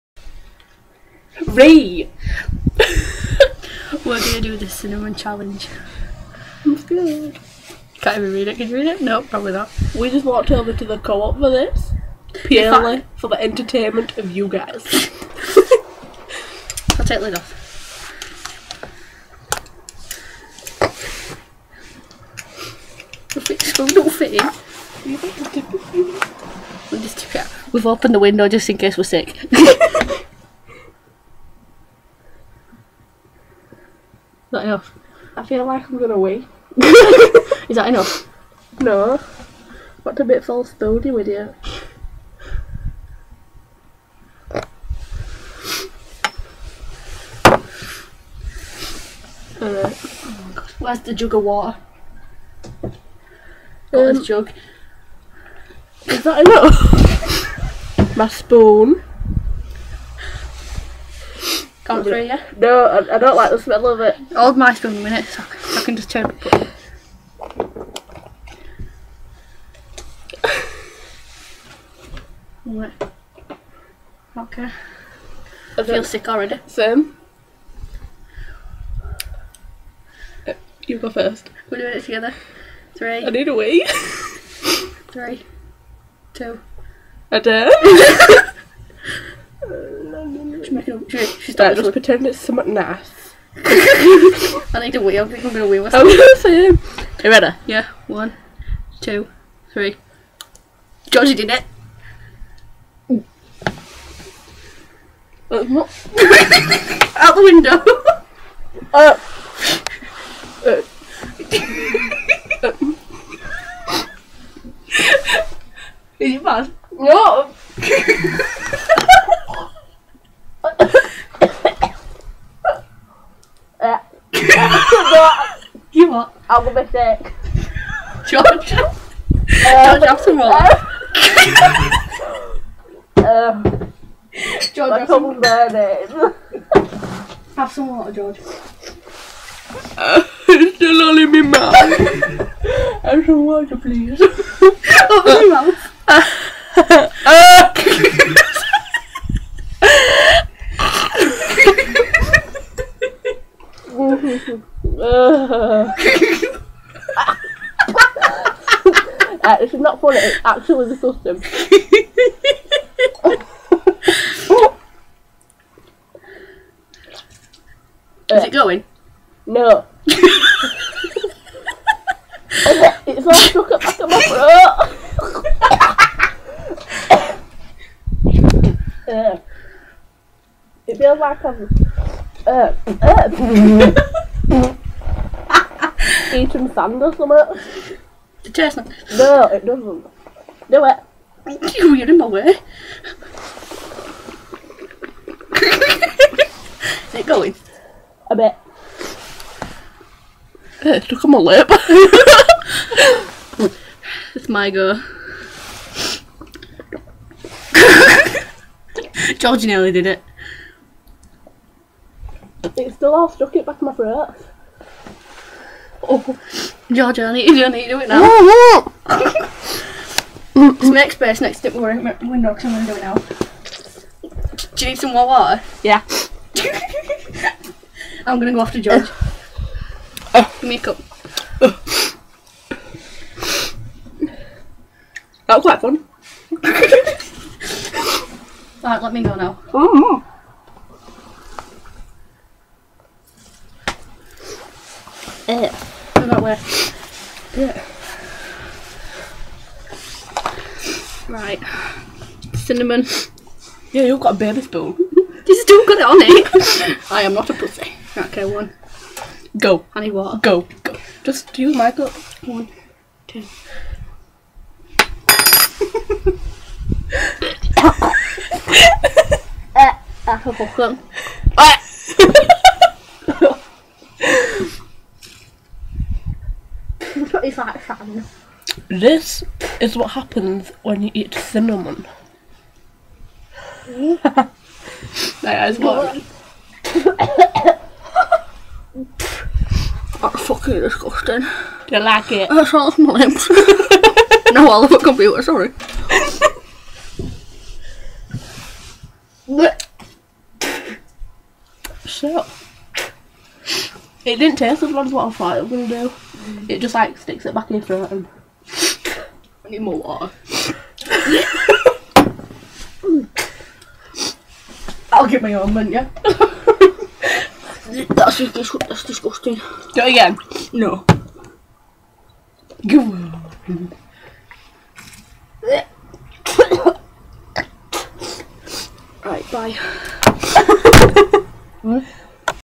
Me and Georgia doing 'The Cinnamon Challenge'.
Well safe to say this is one of the worst things i have experienced in my life, never going it again. Great day though, lol ^.^